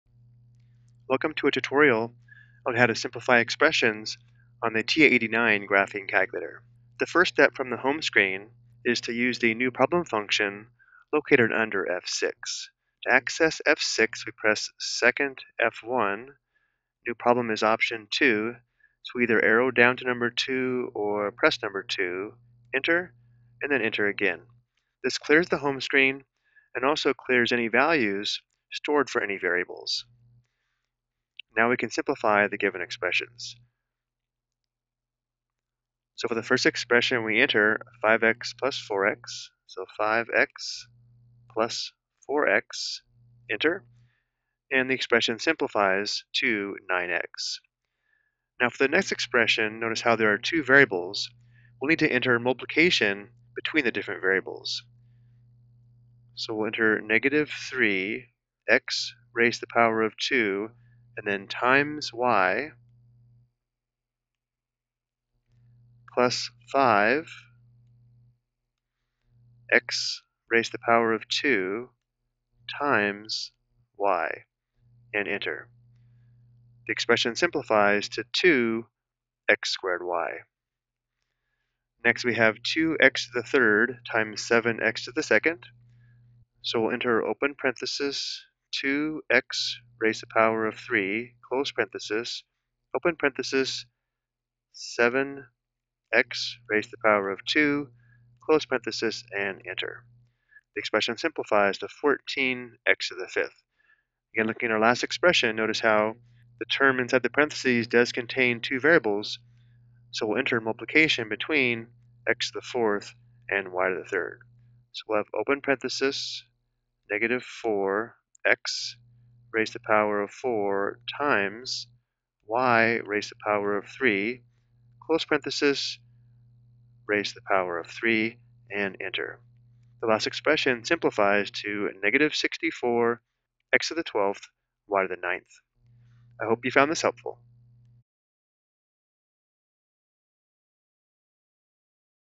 Simplifying Expressions on the TI-89
This video explains how to simplify expressions on the TI-89.